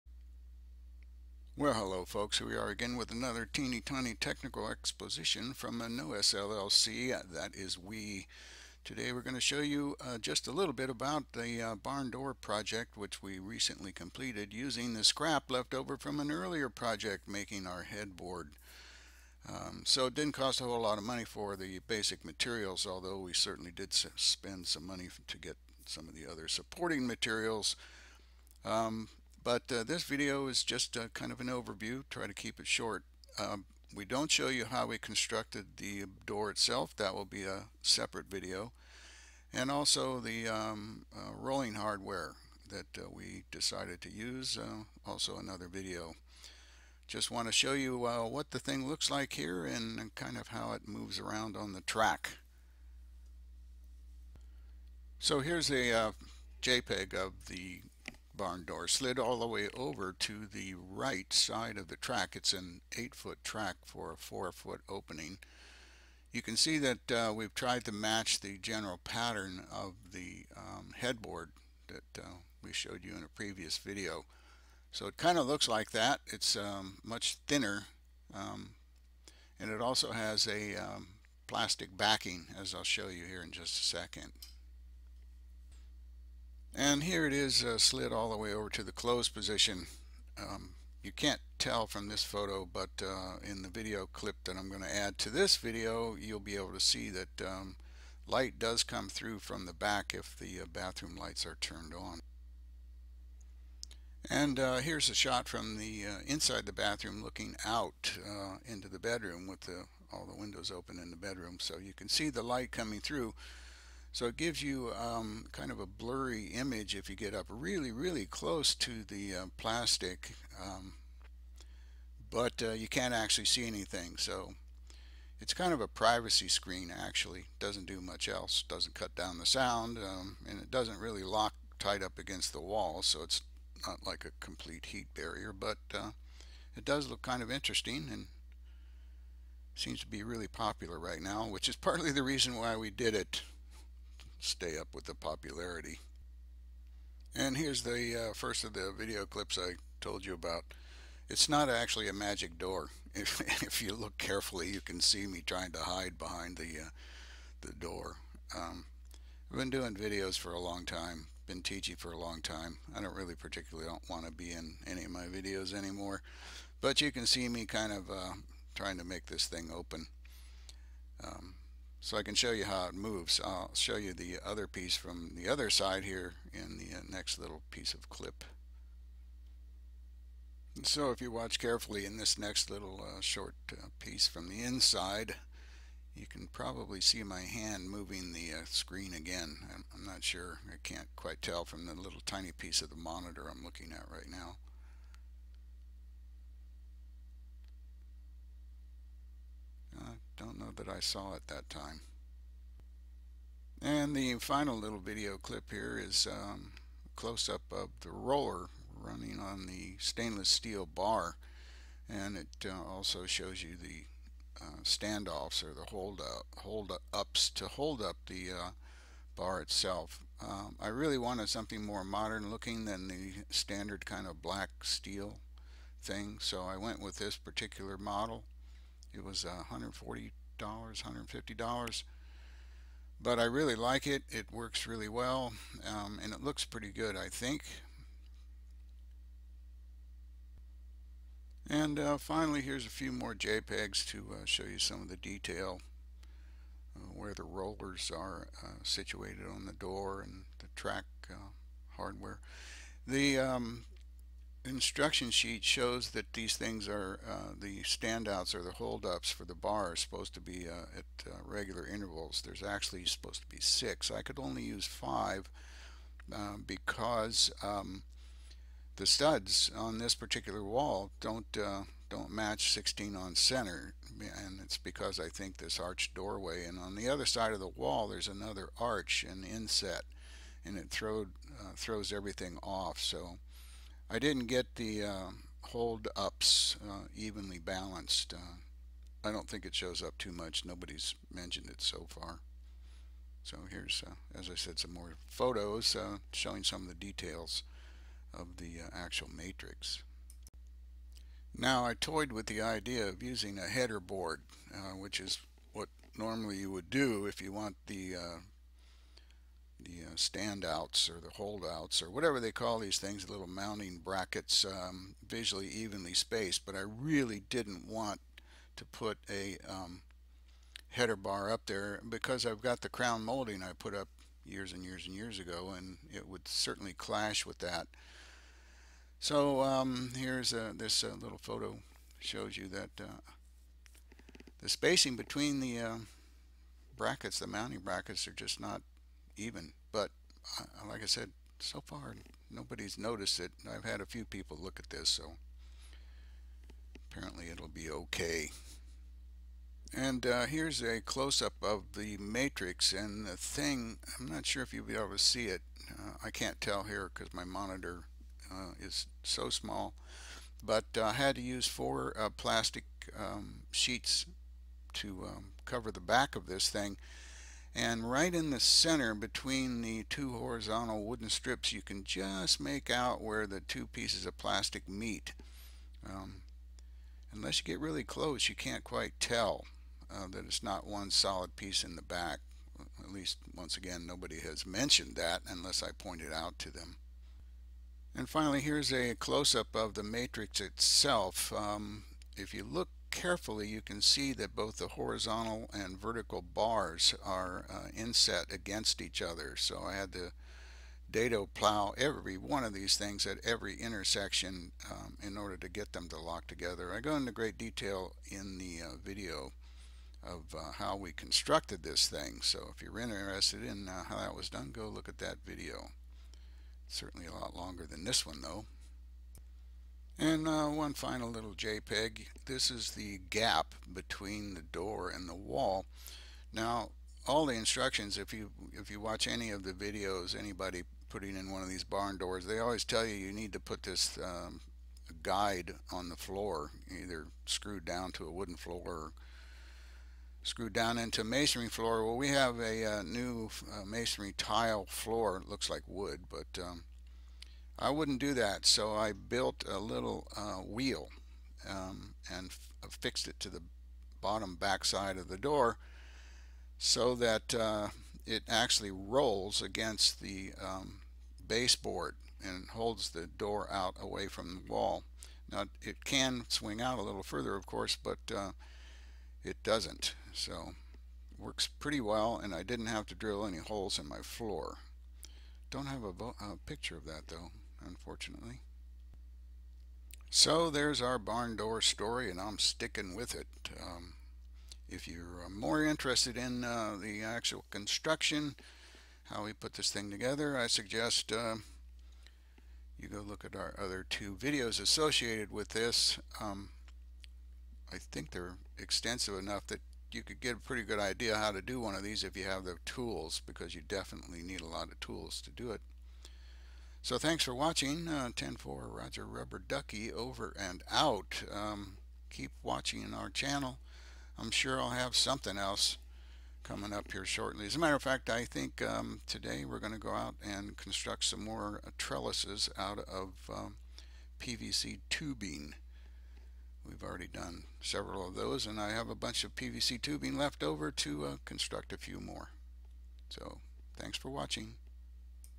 Barn Door Project (Description & Operation)
If not unique, it's at least a very unusual design for an interior barn door. It's made from the leftover lumber from our Head Board Project, some plastic florescent panels and a commercial stainless steel track system. This vid is just an overview. Construction details are in a separate video.